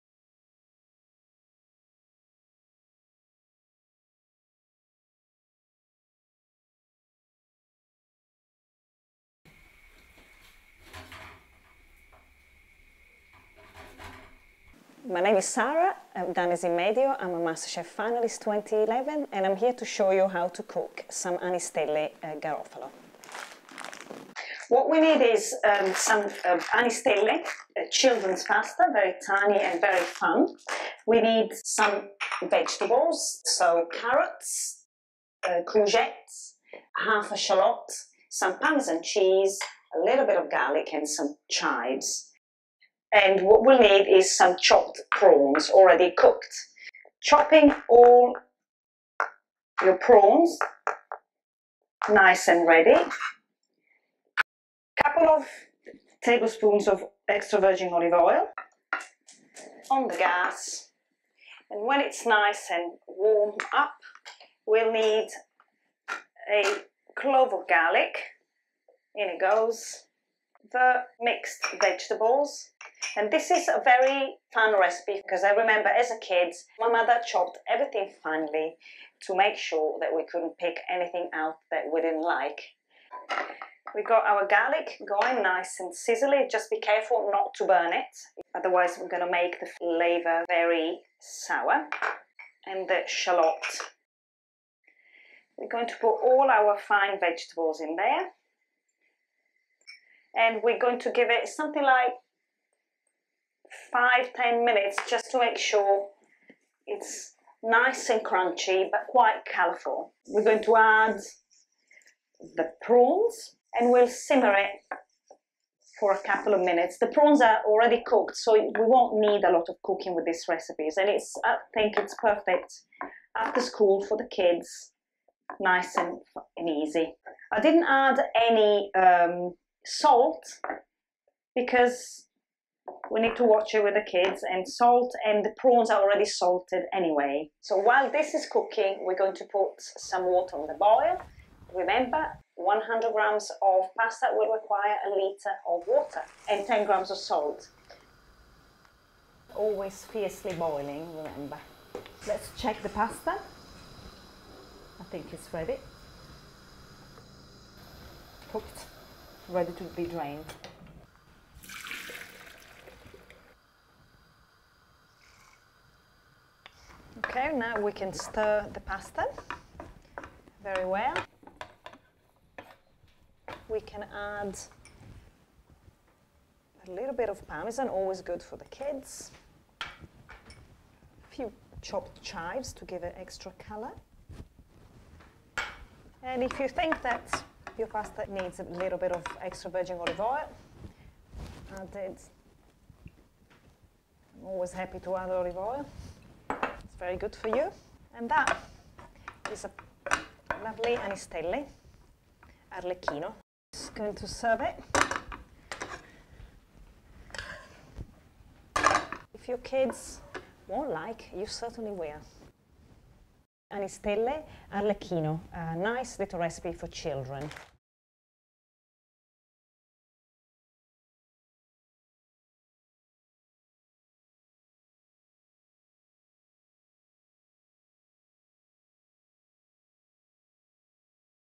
Anistelle Arlecchino (with courgettes, carrots, shallots and small prawns)
This very colourful and fun recipe is named after Arlecchino, the famous clown. This recipe is deliciously healthy, combining the goodness of the vegetables with the balanced proteins of the shellfish.